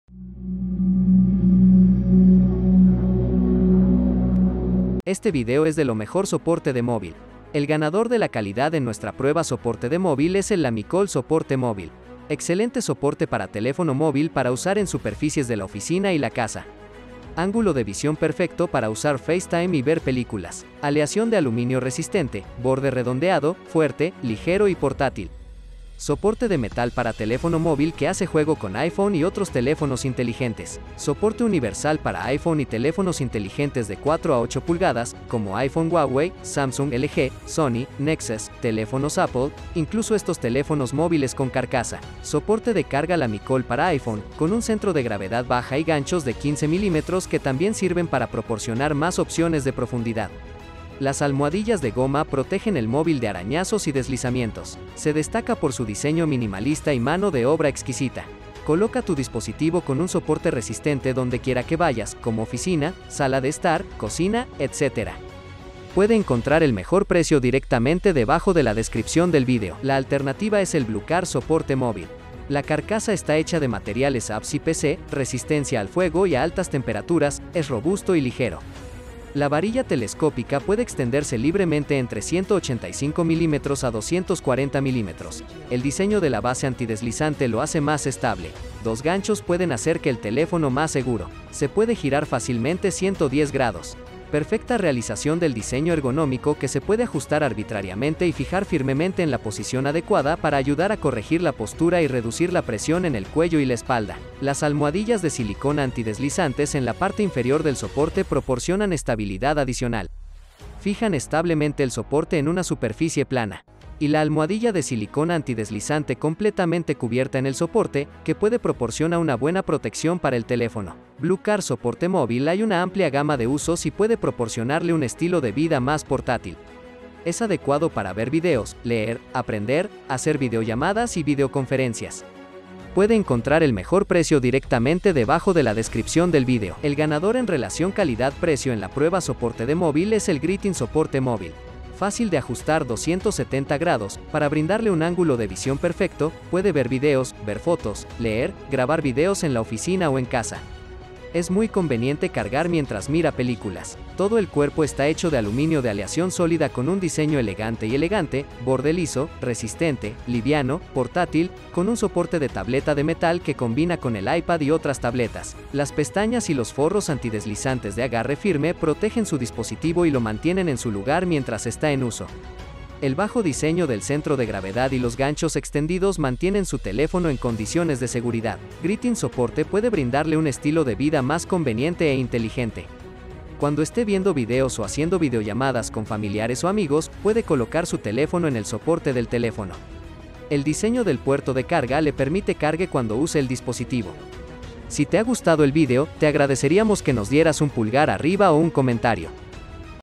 TOP 3 🥇 MEJOR SOPORTE DE MÓVIL ¦ Comparación de Amazon ✓
este video es de lo mejor SOPORTE DE MÓVIL.

El ganador de la calidad en nuestra prueba SOPORTE DE MÓVIL es el Lamicall Soporte Movil.

Excelente soporte para teléfono móvil para usar en superficies de la oficina y la casa. Ángulo de visión perfecto para usar Facetime y ver películas.
Aleación de aluminio resistente, borde redondeado, fuerte, ligero y portátil. Soporte de metal para teléfono móvil que hace juego con iPhone y otros teléfonos inteligentes.
Soporte universal para iPhone y teléfonos inteligentes de 4 a 8 pulgadas, como iPhone Huawei, Samsung LG, Sony, Nexus, teléfonos Apple, incluso estos teléfonos móviles con carcasa.
Soporte de carga Lamicall para iPhone, con un centro de gravedad baja y ganchos de 15 mm que también sirven para proporcionar más opciones de profundidad. Las almohadillas de goma protegen el móvil de arañazos y deslizamientos.
Se destaca por su diseño minimalista y mano de obra exquisita. Coloca tu dispositivo con un soporte resistente dondequiera que vayas, como oficina, sala de estar, cocina, etc.
Puede encontrar el mejor precio directamente debajo de la descripción del vídeo.

La alternativa es el Blukar Soporte Movil.

La carcasa está hecha de materiales ABS y PC, resistencia al fuego y a altas temperaturas, es robusto y ligero. La varilla telescópica puede extenderse libremente entre 185 mm-240 mm; El diseño de la base antideslizante lo hace más estable; Dos ganchos pueden hacer que el teléfono más seguro.
Se puede girar fácilmente 110 °. Perfecta realización del diseño ergonómico que se puede ajustar arbitrariamente y fijar firmemente en la posición adecuada para ayudar a corregir la postura y reducir la presión en el cuello y la espalda.
Las almohadillas de silicona antideslizantes en la parte inferior del soporte proporcionan estabilidad adicional. Fijan establemente el soporte en una superficie plana. Y la almohadilla de silicona antideslizante completamente cubierta en el soporte, que puede proporciona una buena protección para el teléfono.
Blukar soporte móvil hay una amplia gama de usos y puede proporcionarle un estilo de vida más portátil. Es adecuado para ver videos, leer, aprender, hacer videollamadas y videoconferencias.
El ganador en relación calidad-precio en la prueba SOPORTE DE MÓVIL es el Gritin Soporte Movil.

Fácil de ajustar 270°, para brindarle un ángulo de visión perfecto, puede ver videos, ver fotos, leer, grabar videos en la oficina o en casa. Es muy conveniente cargar mientras mira películas.
Todo el cuerpo está hecho de aluminio de aleación sólida con un diseño elegante y elegante, borde liso, resistente, liviano, portátil, con un soporte de tableta de metal que combina con el iPad y otras tabletas.
Las pestañas y los forros antideslizantes de agarre firme protegen su dispositivo y lo mantienen en su lugar mientras está en uso. El bajo diseño del centro de gravedad y los ganchos extendidos mantienen su teléfono en condiciones de seguridad.
Gritin soporte puede brindarle un estilo de vida más conveniente e inteligente. Cuando esté viendo videos o haciendo videollamadas con familiares o amigos, puede colocar su teléfono en el soporte del teléfono. El diseño del puerto de carga le permite Cargue cuando use el dispositivo.